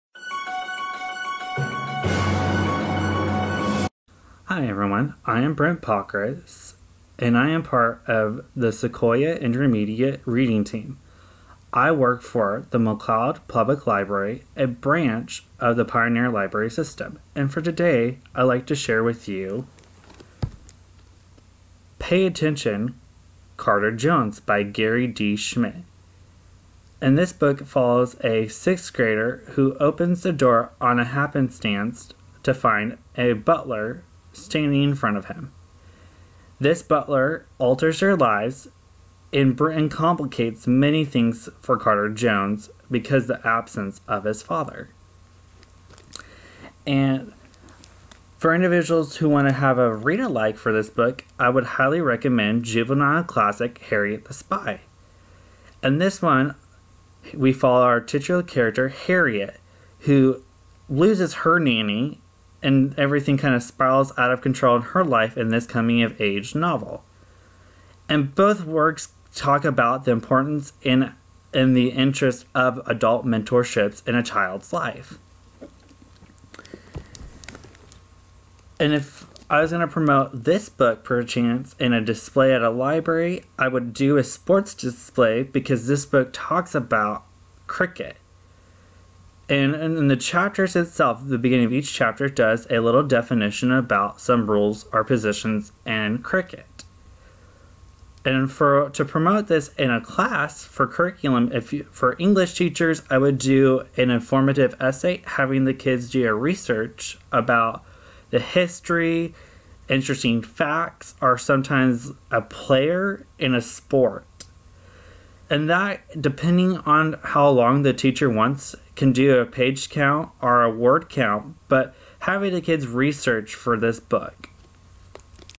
Pay Attention, Carter Jones - 2021 Intermediate Sequoyah Masterlist
Brent Pockrus presents “Pay Attention, Carter Jones” by Gary D. Schmidt, a title on the 2021 Intermediate Sequoyah Masterlist.

Hear from OLA Sequoyah reading team members each Saturday as they highlight a title from the 2021 Masterlists, give some readalike titles, and give suggestions for how to promote that Sequoyah title in schools and libraries!